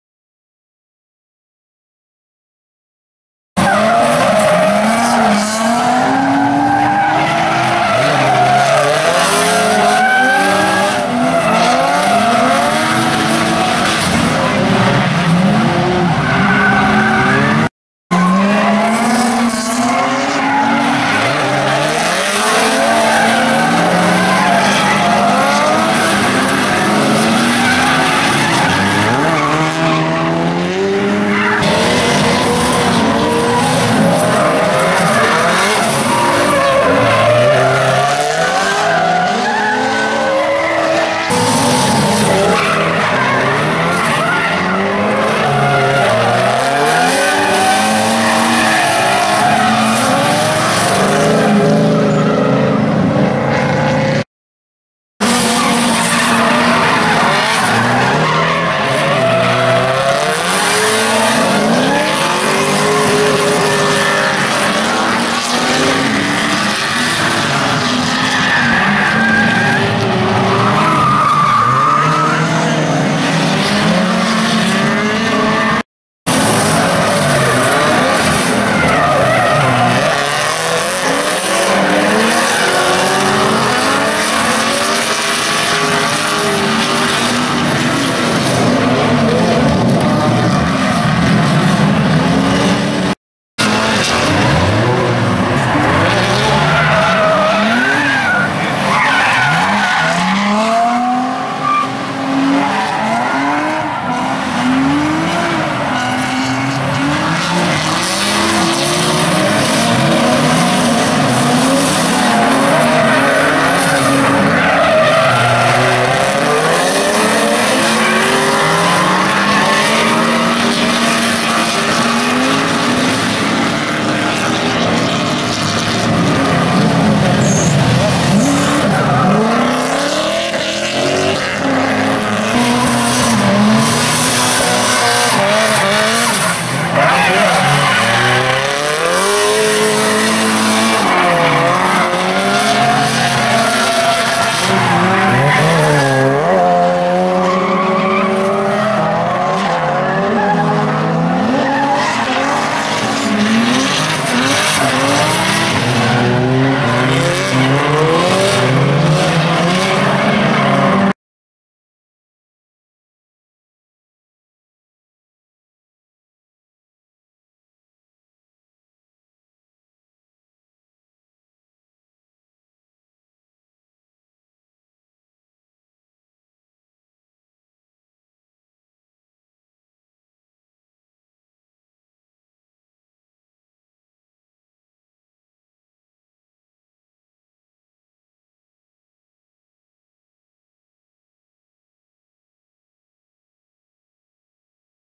Vincent Moore - 8 runs and some photos
September 3, 2016, ITrack Motorsports: Midpond Circuit Drift